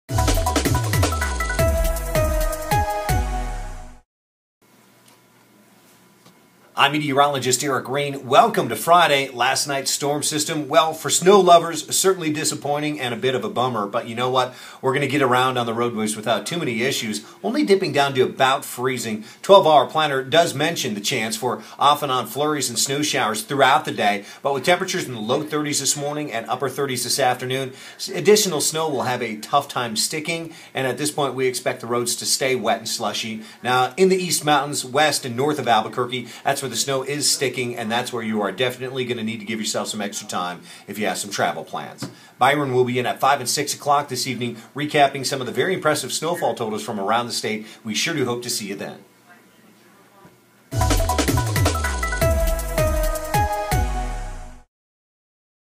Snow sticking in East Mountains
For snow lovers in Albuquerque, Thursday night's storm system was a bit of a bummer.